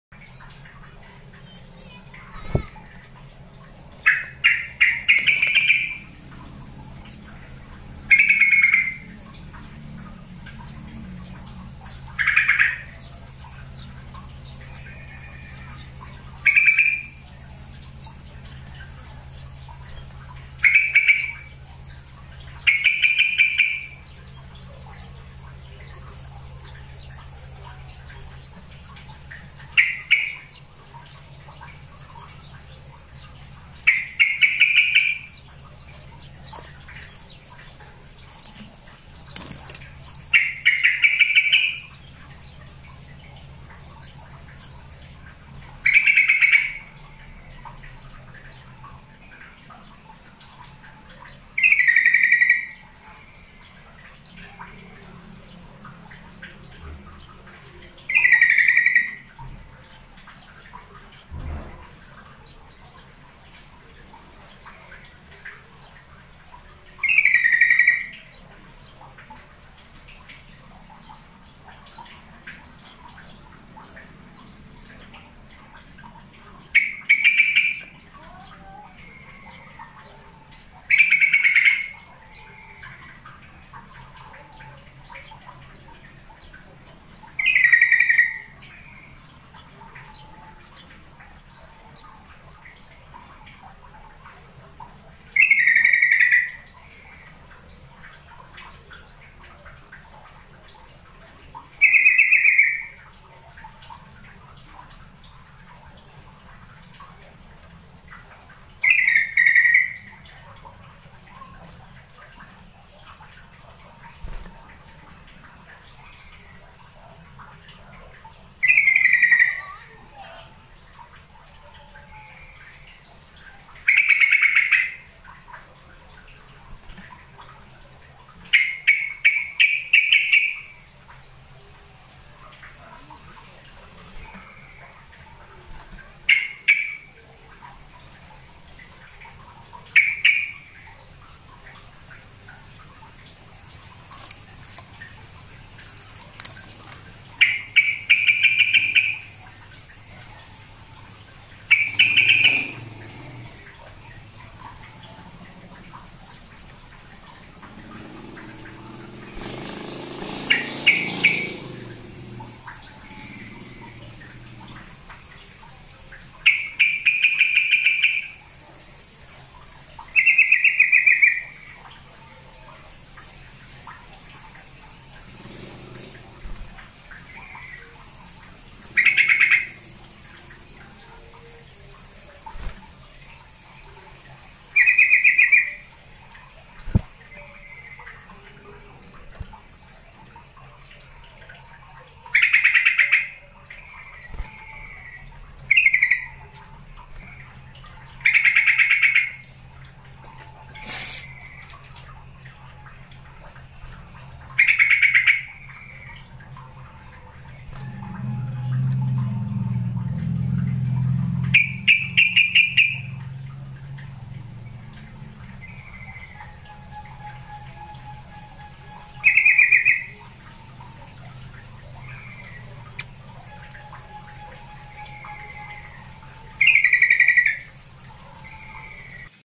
Pancingan suara trucukan agar bunyi | trucuk grabak jadi jinak
Mp3 trucuk pemanggil
Trucukan dari grabagan same jinak mandi sendiri